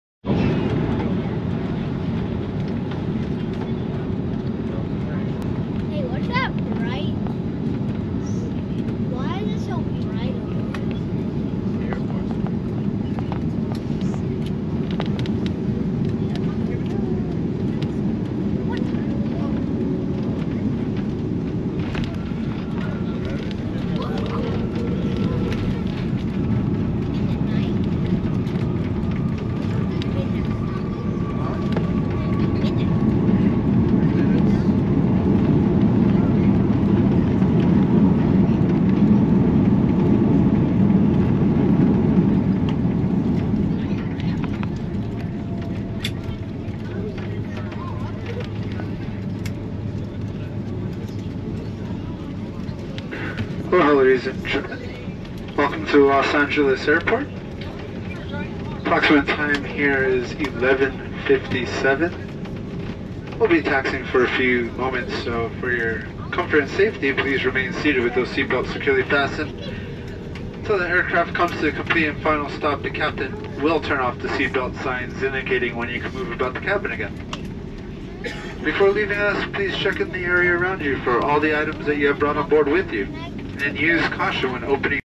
Hawaiian Airlines Airbus A330 Landing In LAX
4K GoPro Footage Of Hawaiian Airlines Airbus A330 Landing In Los Angeles (LAX)
Flight Info
Aircraft / Airbus A330-243
Flight Number / HA62
Flight Time / 5:38 Hrs